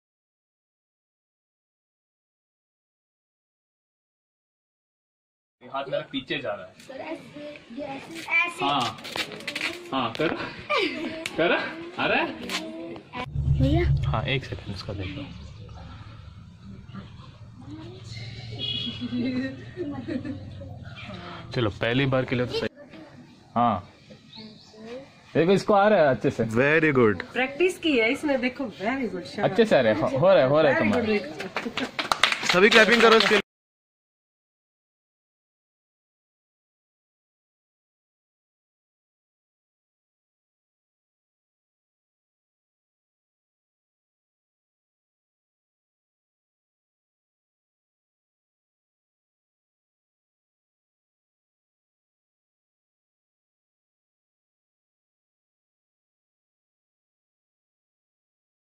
Brain Development Session | Asha Education Center | Joniawas
Here is a glimpse of Brain Development session taken by Brainweave fellows at Asha Education Center at Joniawas on January 20, 2018.